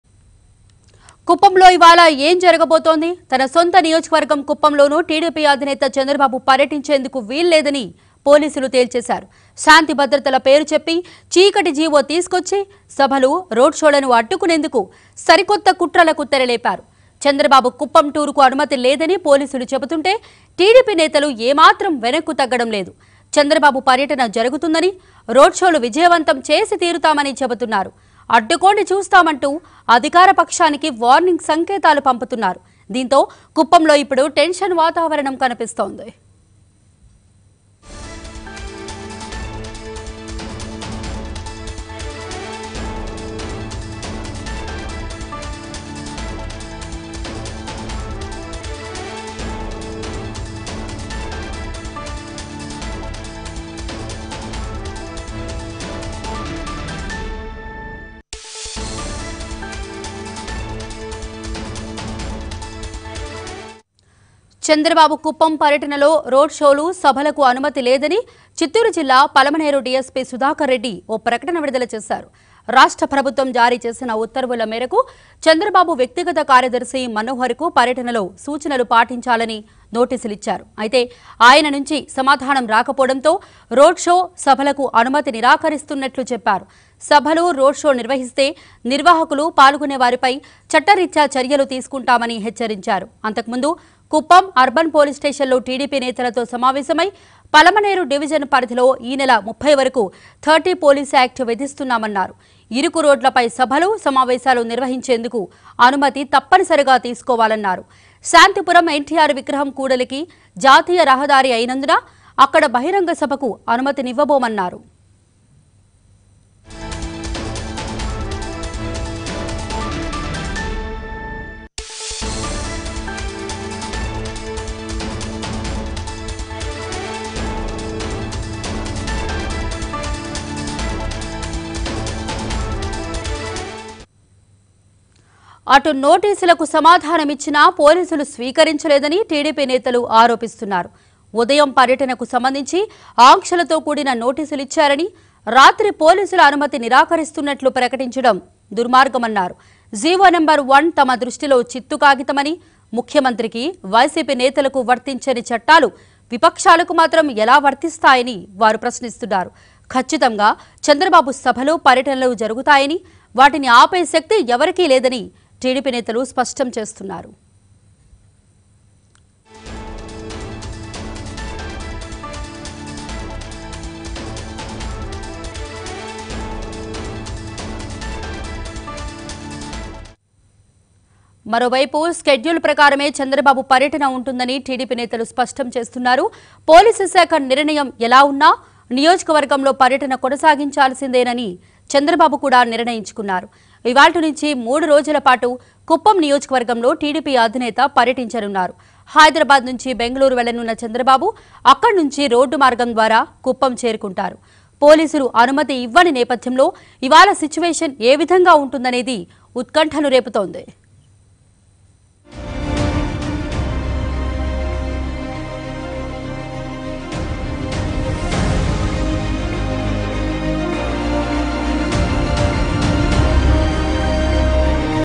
కుప్పంలో హైటెన్షన్..! | High Tension Situation at Kuppam | Chandrababu Naidu Kuppam Tour | TV5 News
అడ్డుకోండి చూస్తాం..!
చంద్రబాబు కుప్పం పర్యటనపై హైటెన్షన్..!